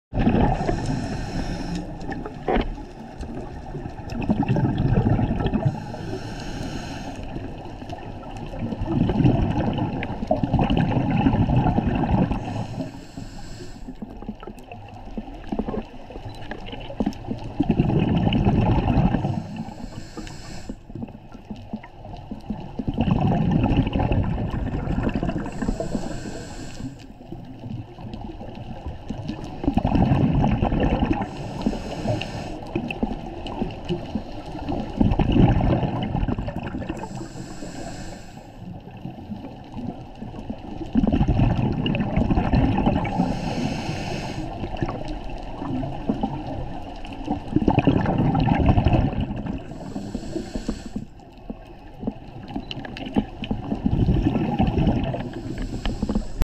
Sea Turtle Blowing Bubbles!
Dive master Megan for Sail Fish Scuba dive shop in Key Largo, Florida caught this rare video of a sea turtle blowing bubble while cleaning it's shell on the Benwood shipwreck!